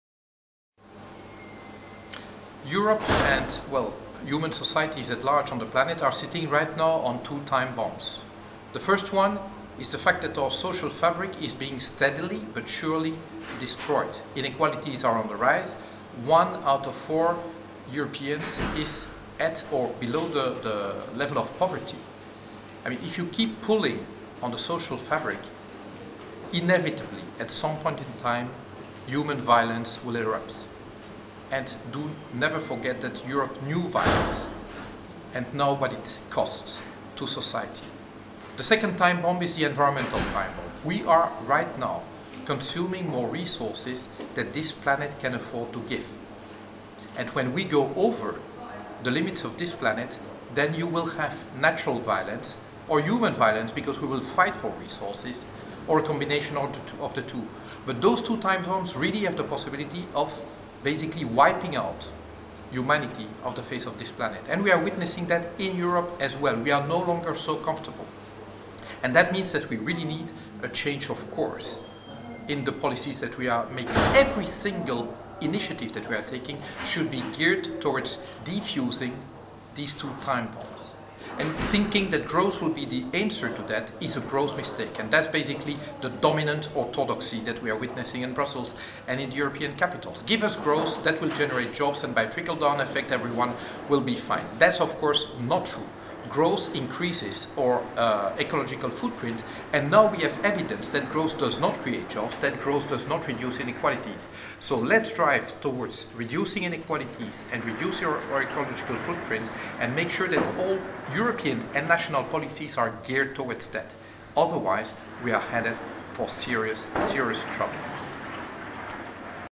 MEP Philippe Lamberts: "We are sitting on two time-bombs"
Philippe Lamberts, Member of the European Parliament, at Caritas Europa's high-level conference "The Growing Crisis in Europe" on 21 February 2013.